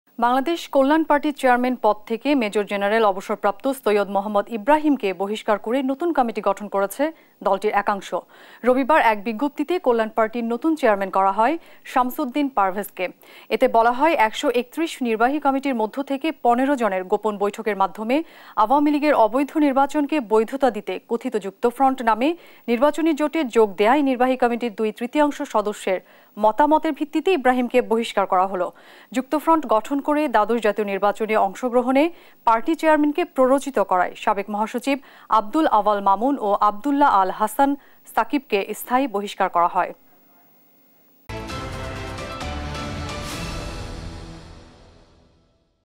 সৈয়দ ইবরাহিমকে বহিষ্কার করে কল্যাণ পার্টির নতুন কমিটি | Syed Muhammad Ibrahim | News | Desh TV
»» Office Address:
42, Shaheed Sangbadik Selina Parveen Sarak, Malibag 1217 Dhaka, Dhaka Division, Bangladesh